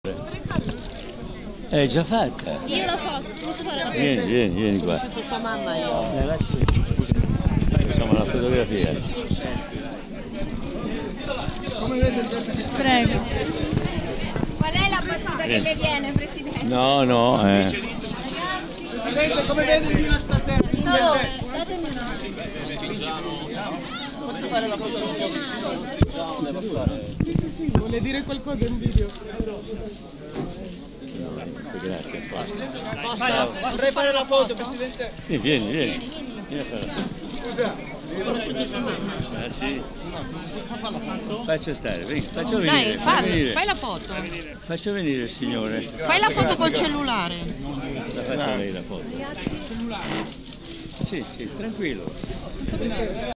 Berlusconi va a votare a Milano acclamato come una star
(Agenzia Vista) - Milano, 4 Marzo 2018 - Prima dell'incidente con la Femen, una marea di gente aveva accolto il Presidente di Forza Italia Silvio Berlusconi al voto questa mattina alla scuola media Dante Alighieri. Courtesy Giuseppe Sciortino